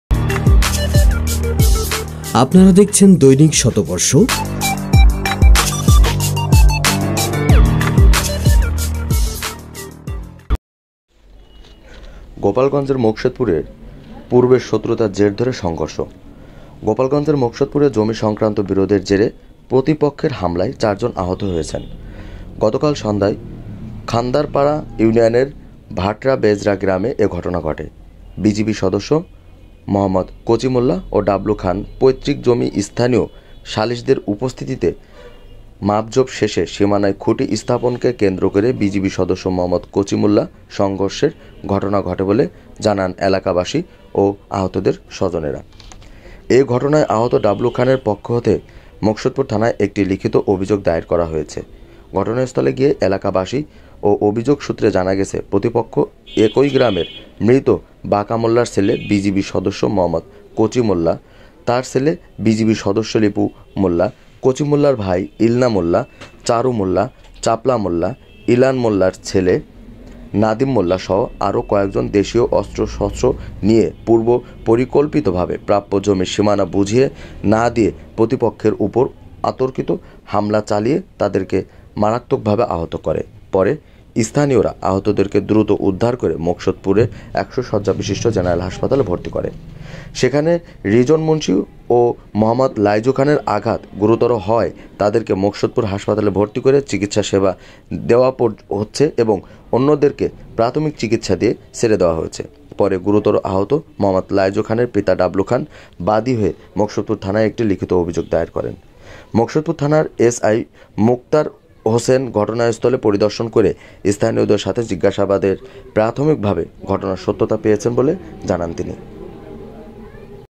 গোপালগঞ্জের মুকসুদপুরে পূর্ব শত্রæতার জের ধরে সংঘর্ষ
গোপালগঞ্জের মুকসুদপুরে জমি সংক্রান্তে বিরোধের জেরে প্রতিপক্ষের হামলায় ৪ জন আহত হয়েছে। গতকাল সন্ধ্যায় খান্দারপাড়া ইউনিয়নের ভাটরা বেজরা গ্রামে এ ঘটনা ঘটে। বিজিবি সদস্য মোঃ কচি মোল্লা ও ডাবলু খান পৈত্রিক জমি স্থানীয় সালিসদের উপস্থিতিতে মাপঝোপ শেষে সীমানায় খুঁটি স্থাপনকে কেন্দ্র করে বিজিবি সদস্য মোঃ কচি মোল্লা সংঘষের ঘটনা ঘটে বলে জানান এলাকাবাসী ও আহতদের স্বজনেরা।
এ ঘটনায় আহত ডাবলু খানের পক্ষে হতে মুকসুদপুর থানায় একটি লিখিত অভিযোগ দায়ের করা হয়েছে।
ঘটনাস্থলে গিয়ে এলাকাবাসী ও অভিযোগ সূত্রে জানা গেছে, প্রতিপক্ষ একই গ্রামের মৃত বাঁকা মোল্লার ছেলে বিজিবি সদস্য মোঃ কচি মোল্লা (৫৫), তার ছেলে বিজিবি সদস্য লিপু মোল্লা, কচি মোল্লার ভাই ইলান মোল্লা, চারু মোল্লা, চাপল মোল্লা, ইলান মোল্লার ছেলে নাদিম মোল্লা সহ আরও কয়েকজন দেশীয় অস্ত্রশস্ত্র নিয়ে পূর্ব পরিকল্পিতভাবে প্রাপ্য জমির সীমানা বুঝিয়ে না দিয়ে প্রতিপক্ষের ওপর অতর্কিত হামলা চালিয়ে তাদেরকে মারাত্মকভাবে আহত করে। পরে স্থানীয়রা আহতদেরকে দ্রæত উদ্ধার করে মুকসুদপুর ১০০ শয্যা বিশিষ্ট জেনারেল হাসপাতালে ভর্তি করে।
সেখানে রিজন মুন্সী ও মোঃ লাইজু খানের আঘাত গুরুতর হওয়ায় তাদেরকে মুকসুদপুর হাসপাতলে ভর্তি করে চিকিৎসাসেবা দেওয়া হচ্ছে এবং অন্যদেরকে প্রাথমিক চিকিৎসা দিয়ে ছেড়ে দেওয়া হয়েছে। পরে গুরুতর আহত মোঃ লাইজু খানের পিতা ডাবøু খান বাদী হয়ে মুকসুদপুর থানায় একটি লিখিত অভিযোগ দায়ের করেন।
মুকসুদপুর থানার এসআই মুক্তার হোসেন ঘটনাস্থল পরিদর্শন করে স্থানীয়দের সাথে জিজ্ঞাসাবাদে প্রাথমিকভাবে ঘটনার সত্যতা পেয়েছে